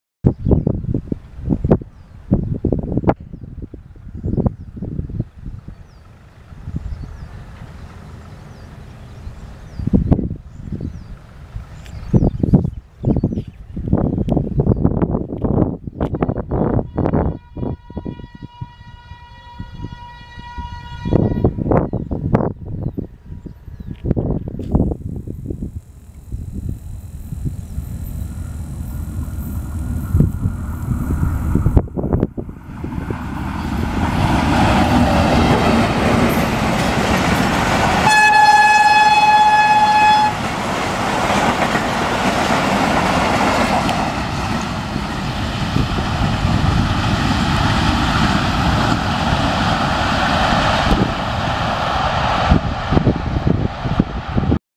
Fastest AGE-30-6011 (Badar / Ghauri Express) 117 Km/hour
Morning delight, Badar Express was coming at time on high speed. Just shoot outside Lahore.